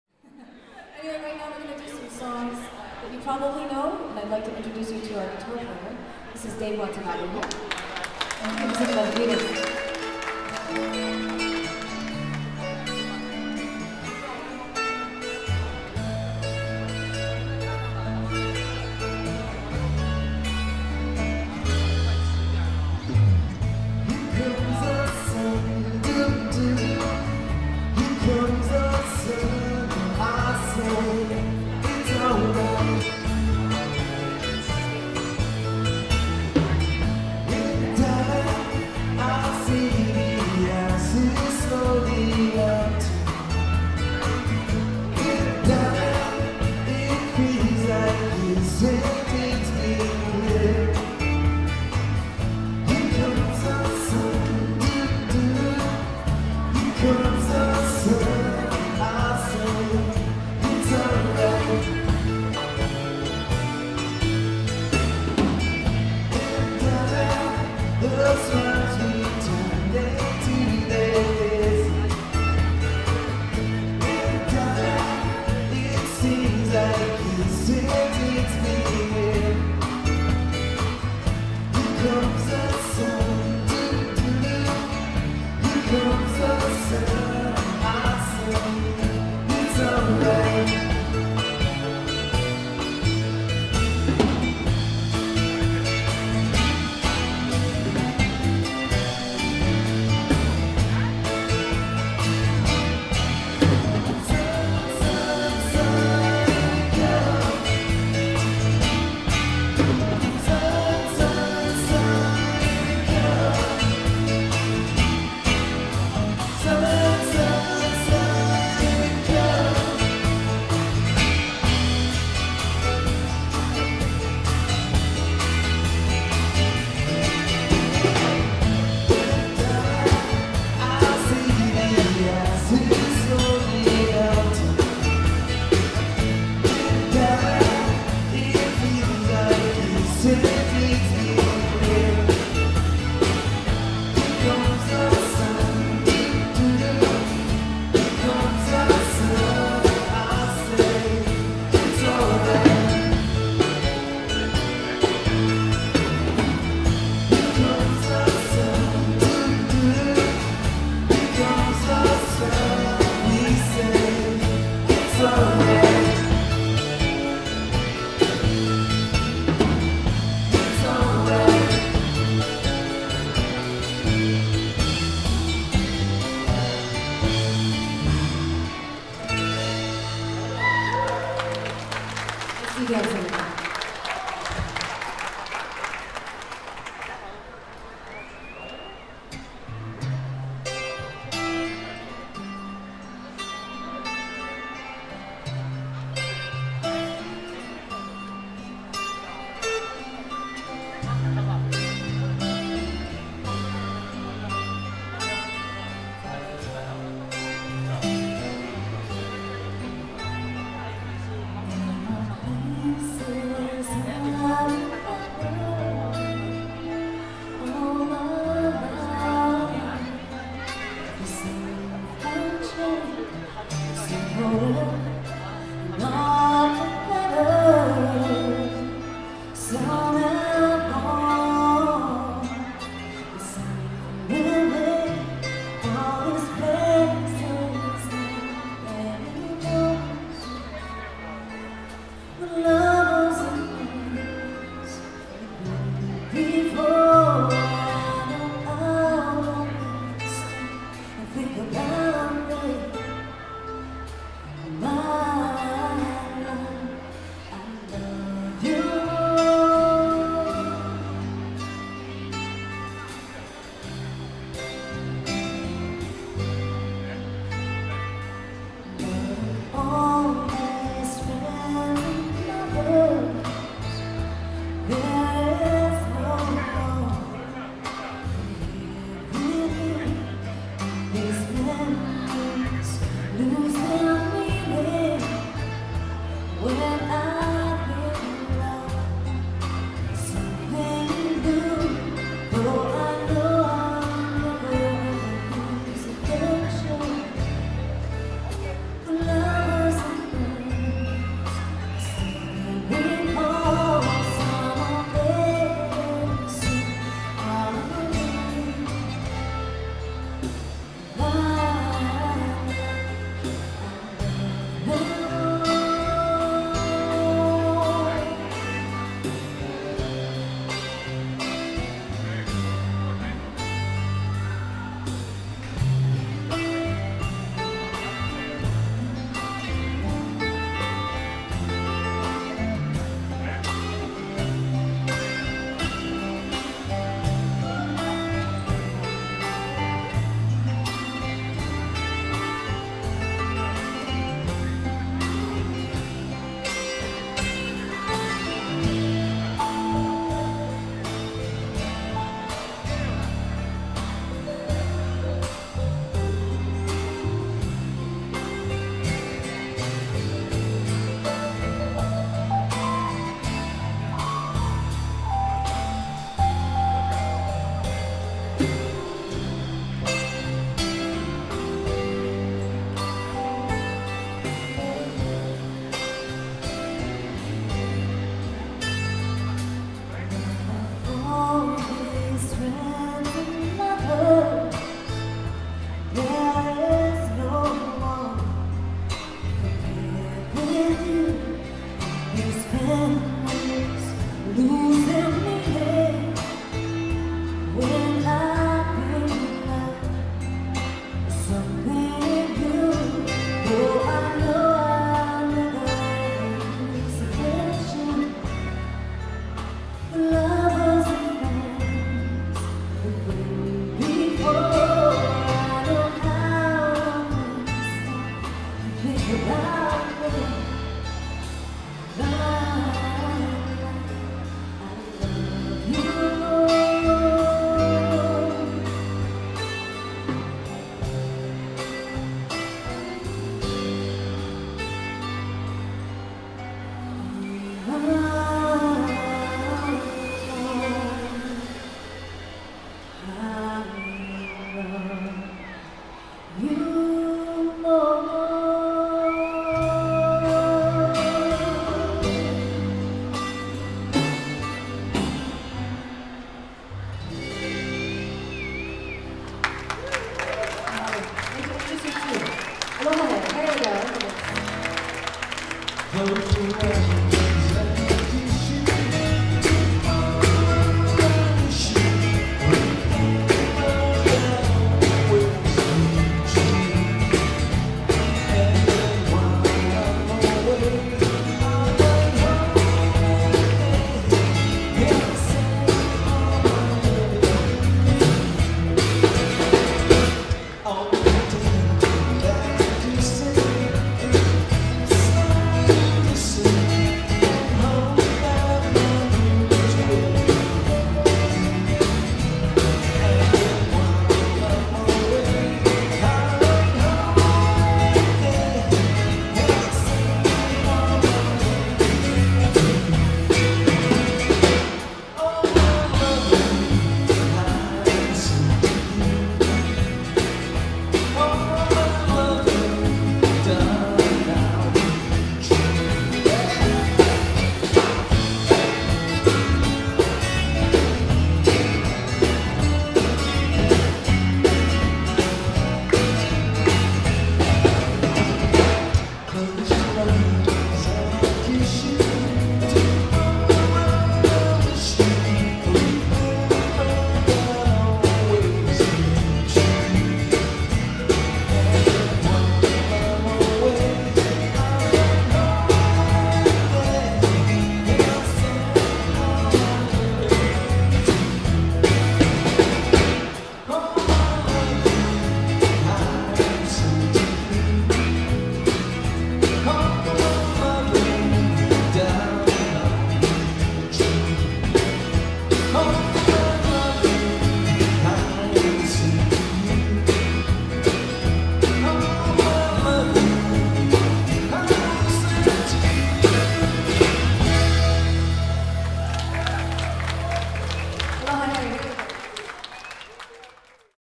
Sunway Live @ Centerstage
Sunway and her band performing a Beatles medley during Shop a Lea.